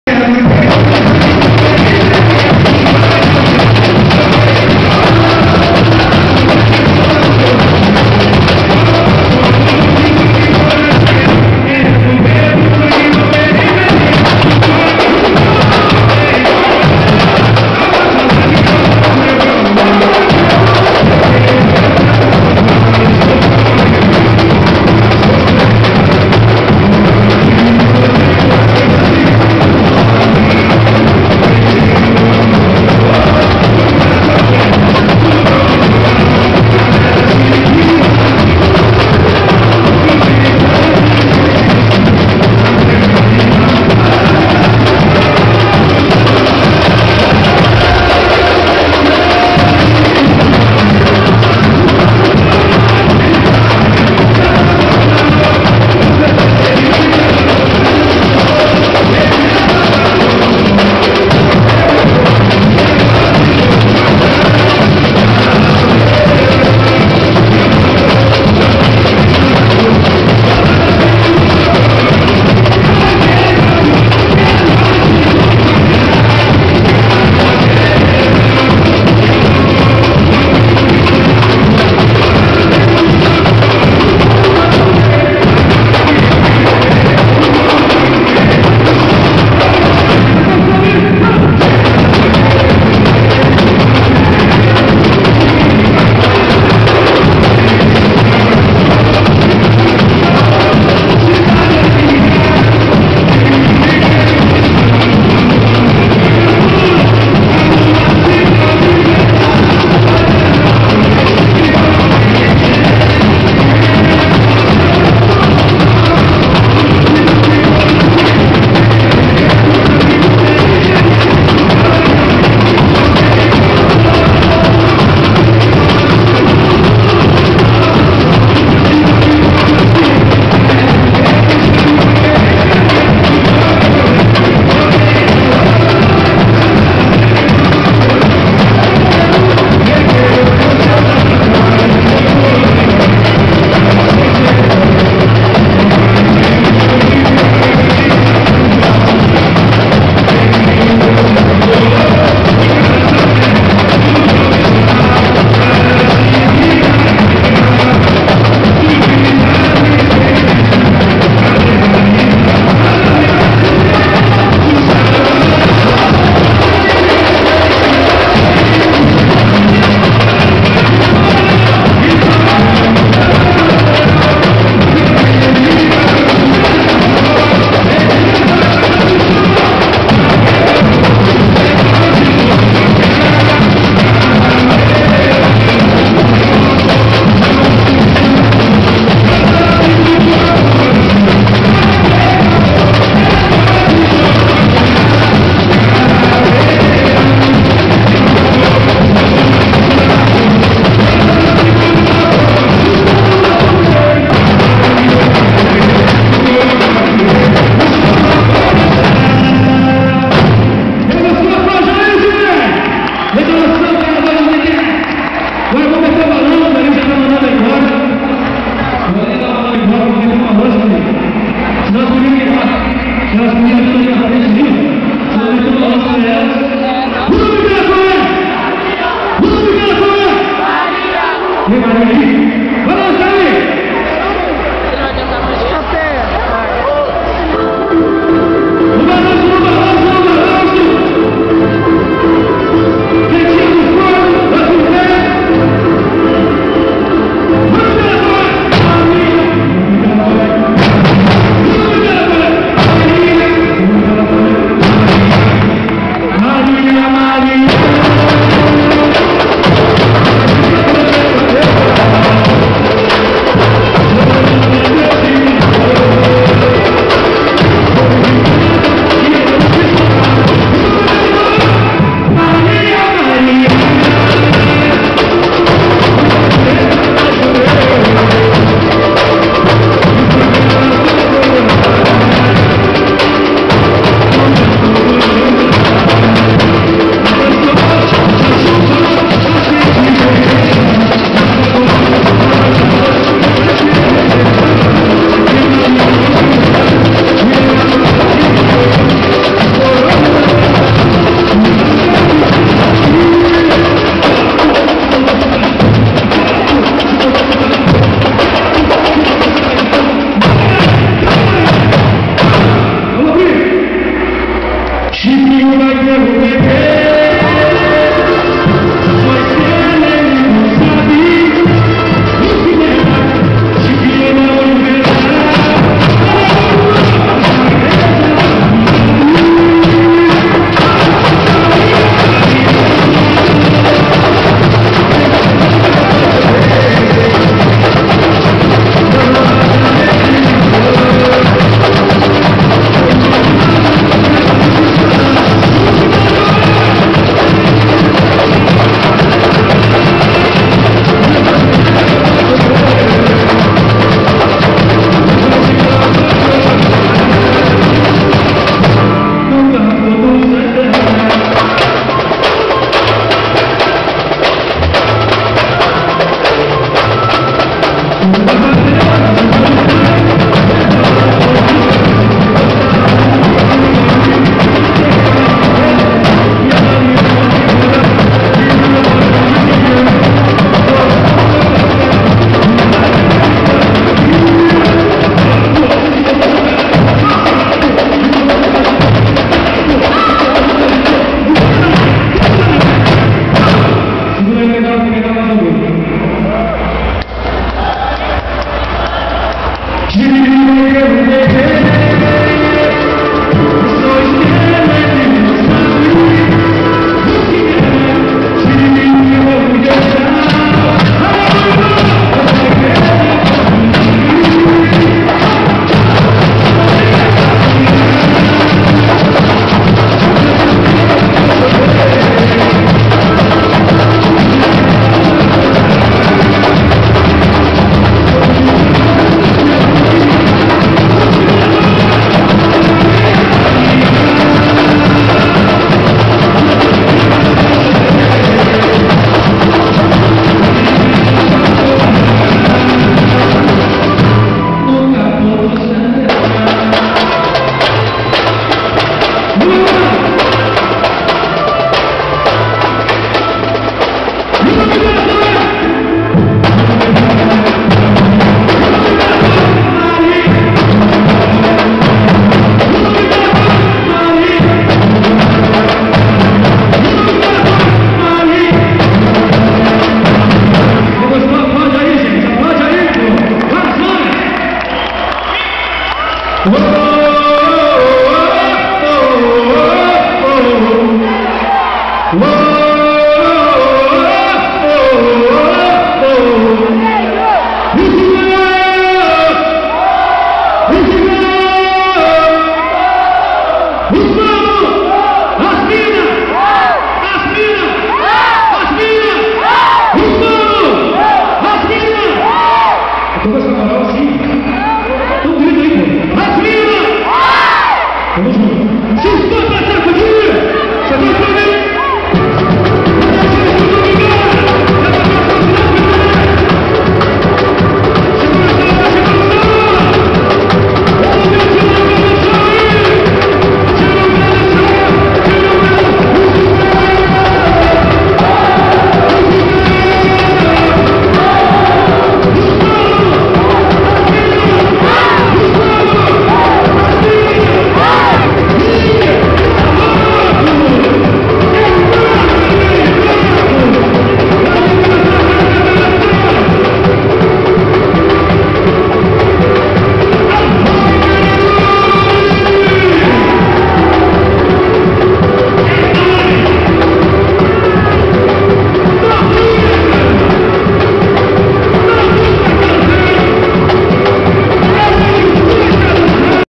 Imperadores do Samba 2010
Show dos Imperadores do Samba com o samba-enredo carnaval 2010 na feijoada realizada pela a Associação das Entidades Carnavalescas de Porto Alegre e do Rio Grande do Sul